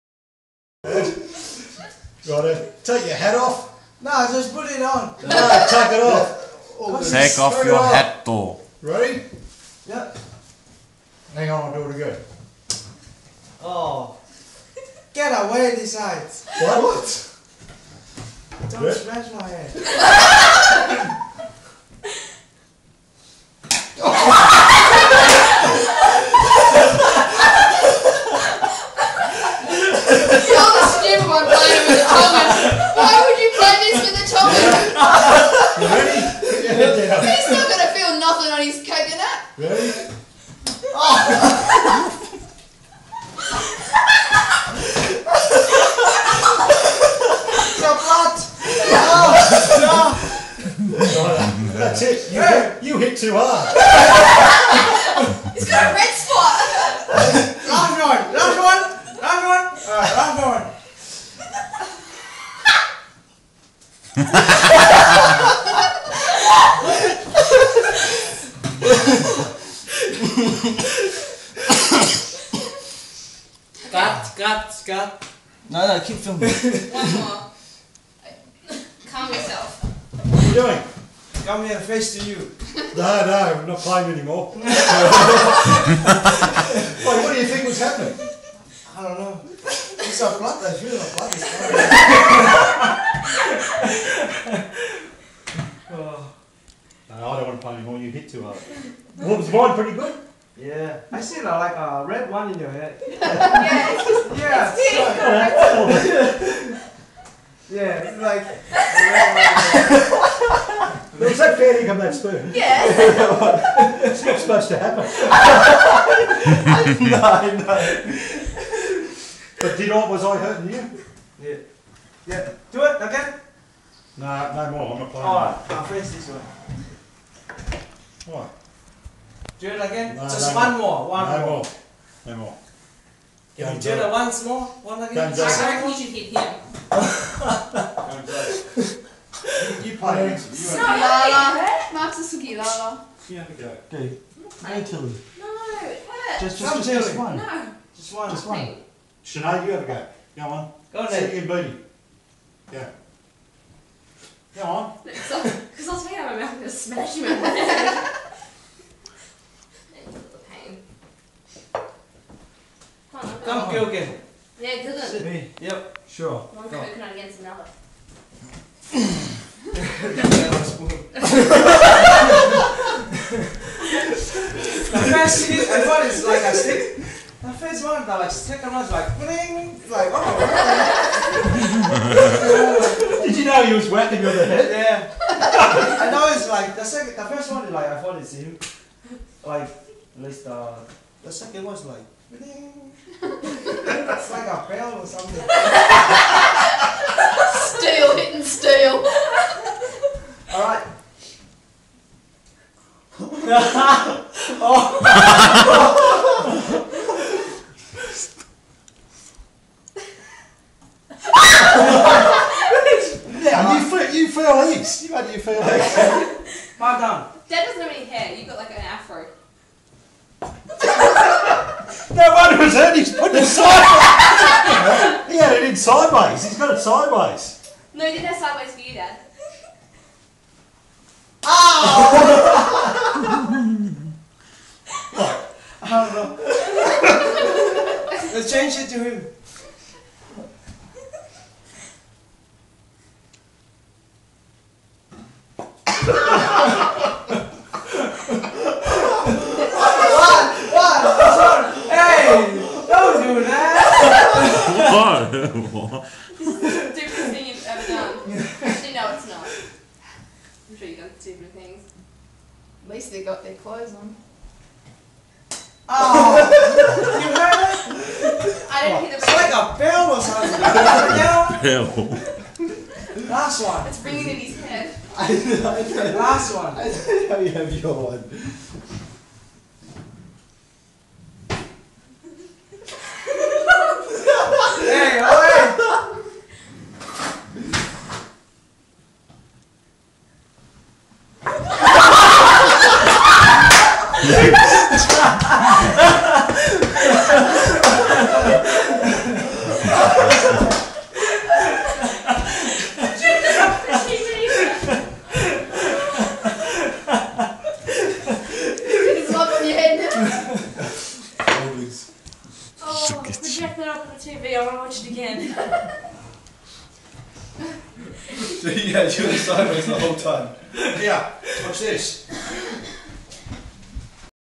Confused Tongan & the spoon game
Elders from my old ward in Bendigo playing the spoon smacking game